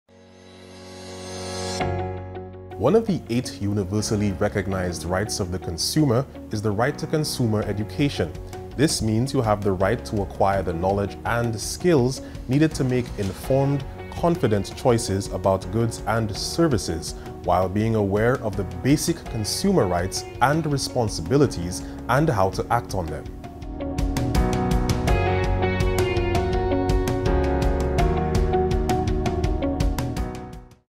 Consumer Affairs Department - The Right to Consumer Education
One of the eight universally-recognized rights of the consumer is the Right to Consumer Education. This means the right to information needed to make informed choices.